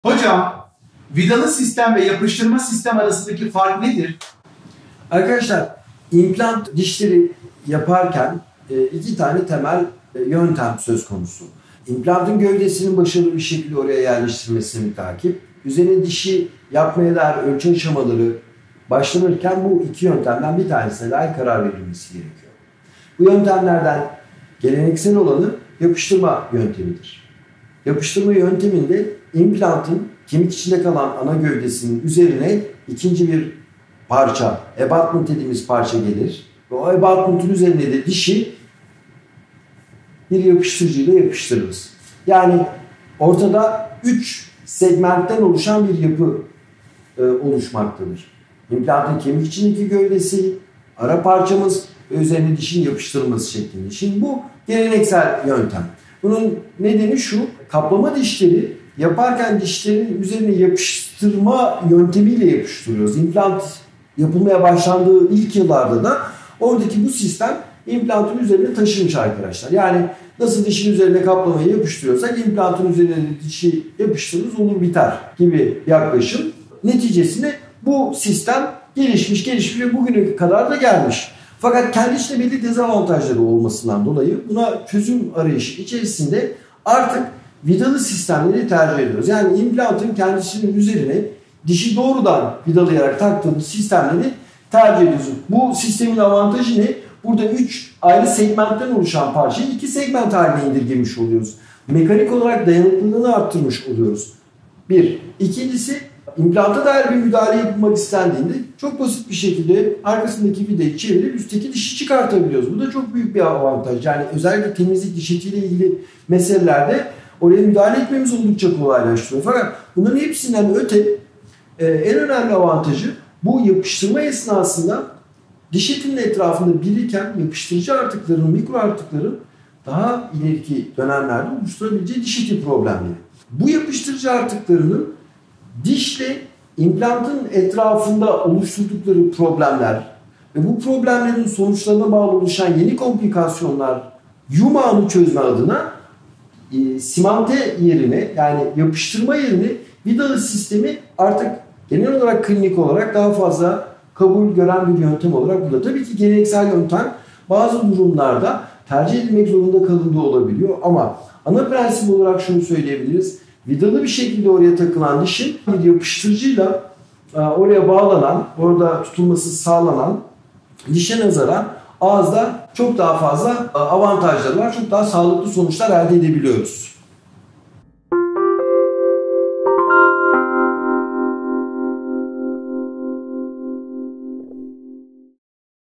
Vİdalı Sistem Ve Yapıştırma Sistem Arasındaki Fark Nedir?
İmplant dişleri yaparken 2 tane temel yöntem söz konusu, implantın gövdesinin başarılı bir sekilde oraya yerleştirilmesine müteakip üzerine dişi yapmaya dair ölçü aşamaları başlanırken bu 2 yöntemden bir tanesine karar verilmesi gerekiyor. Bu yöntemlerden geleneksel olanı yapıştırma yöntemidir. Yapıştırma yönteminde implantın kemik içinde kalan ana gövdesi üzerine 2. Bir parça abutment dediğimiz parça gelir ve o abutmentin üzerine de dişi bir yapıştırıcıyla yapıştırırız yani ortada 3 segmentten oluşan bir yapı oluşmakta ; implantın kemik içindeki gövdesi,  ara parçamız ve üzerine dişin yapıştırılması seklinde bu geleneksel yöntem. Bunun nedeni şu, kaplama dişleri yaparken dişlerin üzerine yapıştırma yöntemiyle yapıştırıyoruz implant yapılmaya başlandığı ilk yıllarda da ordaki bu sistem implantın üzerine taşımış yani nasıl dişin üzerine kaplamayı yapıştırıyorsak implantın üzerine de dişi yapıştırırız olur biter gibi yaklaşım neticesinde bu sistem gelişmiş gelişmiş ve bugüne kadar da gelmiş fakat kendi içinde dezavantajları olmasından dolayı buna çözüm arayışı içersinde artık vidalı sistemleri tercih ediyoruz yani implantın kendisinin üzerine dişi doğrudan vidalayarak taktığımız sistemleri tercih ediyoruz bu sistemin avantajı ne? 3 segmentten oluşan parçayı burda 2 segmente indirmiş oluyoruz mekanik olarak dayanıklılığını artırmış oluyoruz , implanta dair bir müdehale yapılmak istendiğinde çok basit bir sekilde arkasındaki vidayı çevirip üstteki dişi çıkartabiliyoruz bu da çok büyük bir avantaj özellikle diş etiyle ilgili meselelerde oraya müdehale etmemizi oldukça kolaylaştırıyor hatta bunların hepsinden öte en önemli avantajı  yapıştırma esnasında diş etinin etrafında biriken yapıştırıcı artıklarının daha ileriki dönemlerde oluşturabileceği diş eti problemleri.  Bu yapıştırıcı artıklarının dişle implantın etrafında oluşturdukları problemler ve bu problemlerin sonuçlarına bağlı oluşan yeni komplikasyonlar yumağını çözme adına simante yerine yani yapıştırma yerine vidalı sitemi artık genel olarak kabul gören bir yöntemdir tabii ki geleneksel yöntem bazı durumlarda tercih edilmek zorunda kalınabiliyor ama ana prensip olarak şunu söyleyebiliriz vidalı bir sekilde oraya takılan dişin bir yapıştırıcıyla oraya bağlanan oraya tutunması sağlanan dişe nazaran ağızda çok daha fazla avantajları var çok daha sağlıklı sonuçlar elde edebiliyoruz.